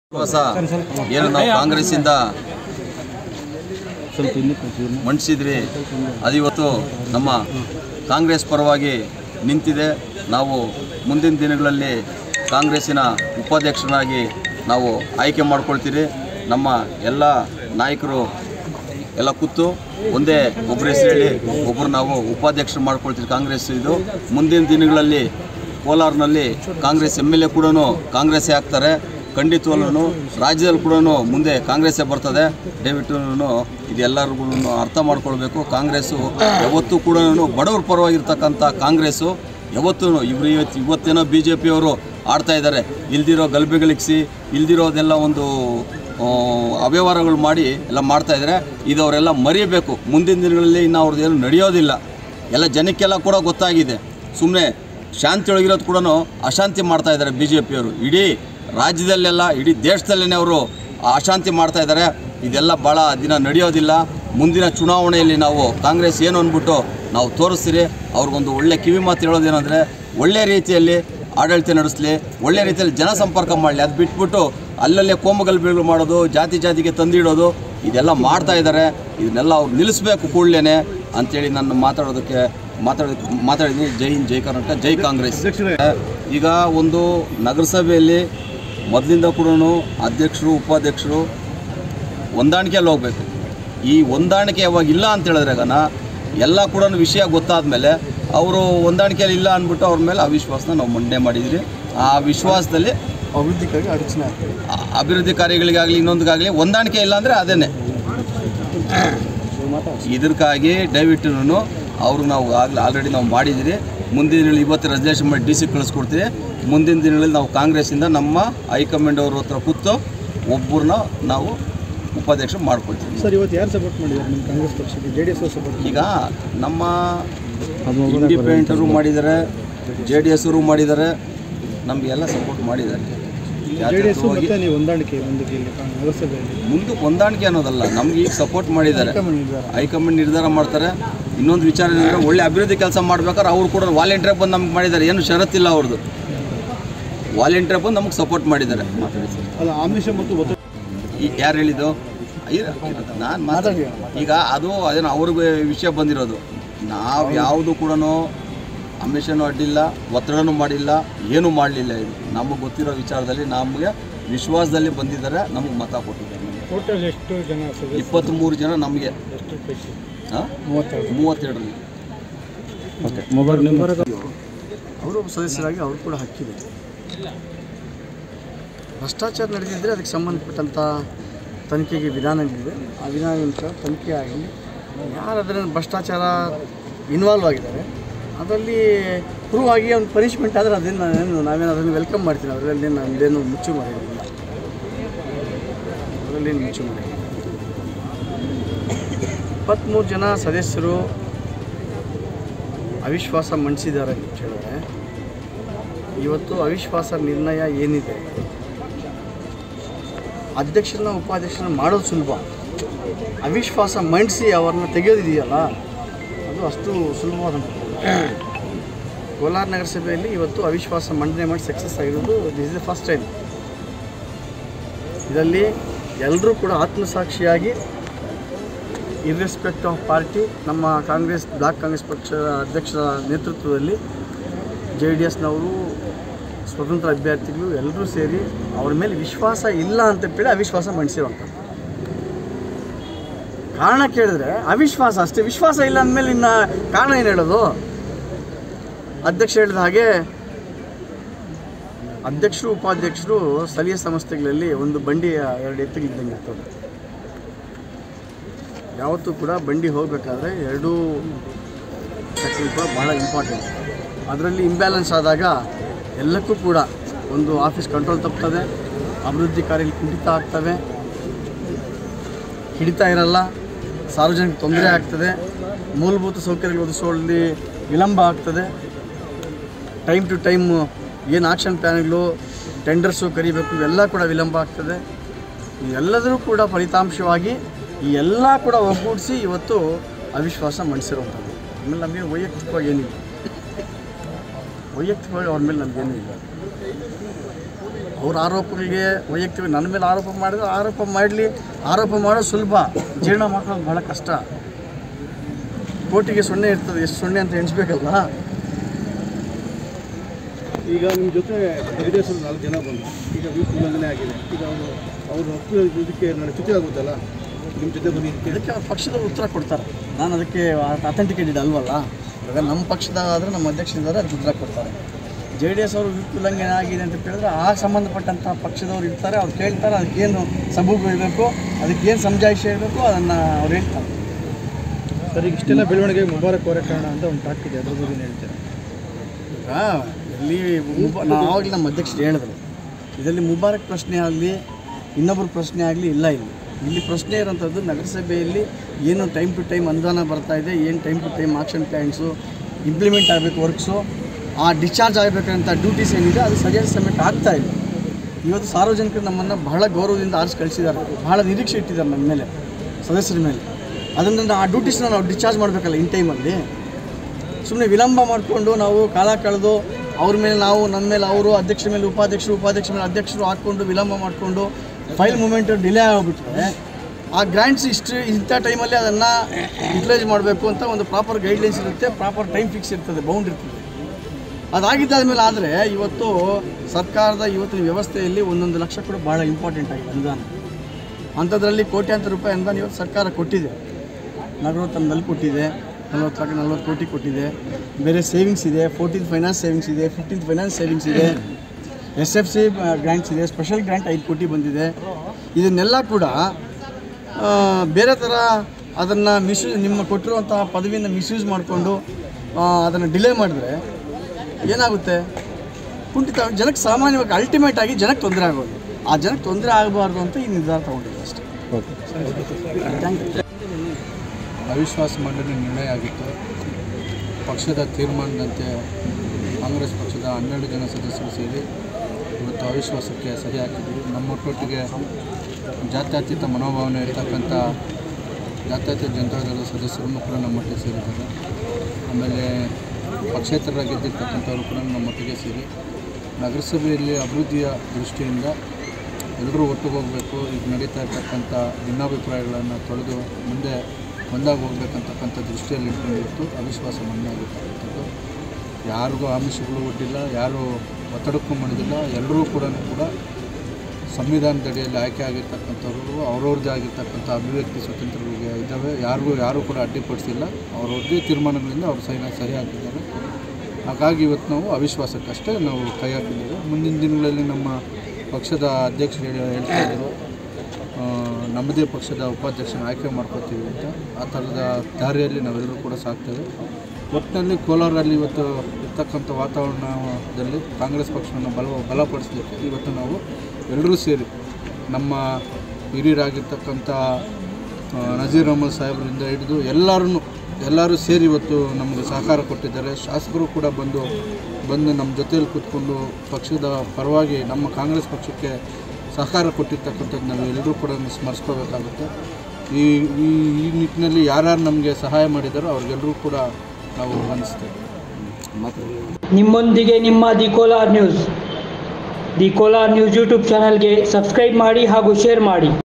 ಮುಂದಿನ ದಿನಗಳಲ್ಲಿ ಕೋಲಾರ ನಗರಸಭೆಯಲ್ಲಿ ಕಾಂಗ್ರೆಸ್ ನ ಉಪಾಧ್ಯಕ್ಷ : ಪ್ರಸಾದ್ ಬಾಬು. | The Kolar News
ಮುಂದಿನ ದಿನಗಳಲ್ಲಿ ಕೋಲಾರ ನಗರಸಭೆಯಲ್ಲಿ ಕಾಂಗ್ರೆಸ್ ನ ಉಪಾಧ್ಯಕ್ಷ : ಪ್ರಸಾದ್ ಬಾಬು.